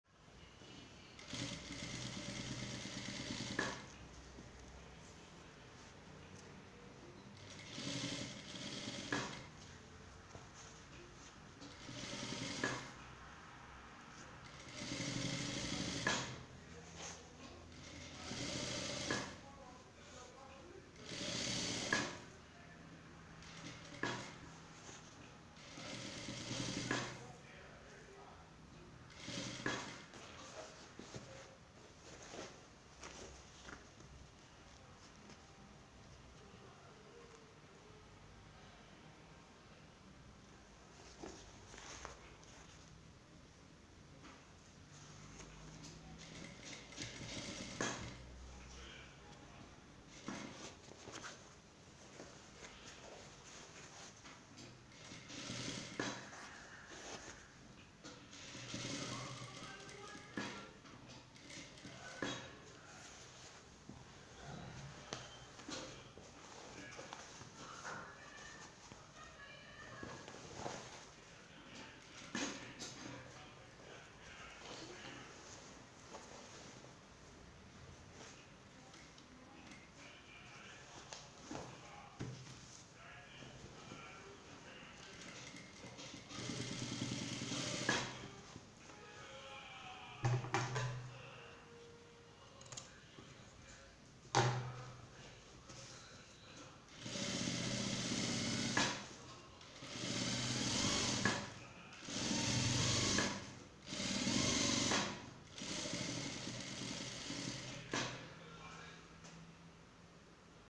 الخطوات الاولى *لخياطة قفة لتسوق #
طريقة خياطة قفة تسوق في الخطوات الاولى بكل خفة للمبتدئات #